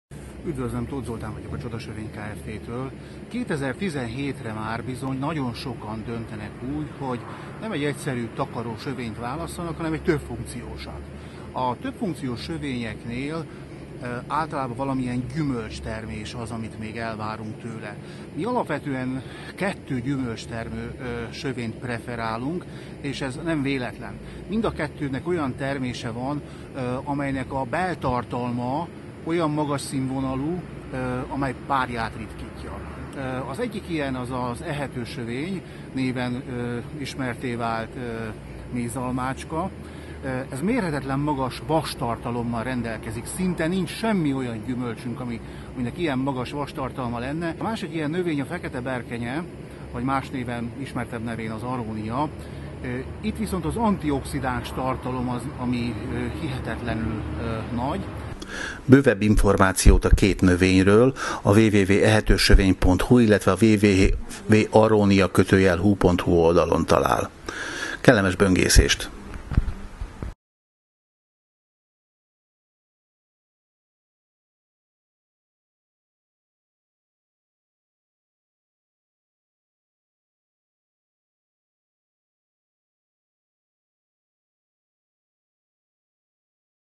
Többfunkciós sövények
Két többfunkciós sövényt is bemutatunk. Részletek : a és a oldalon.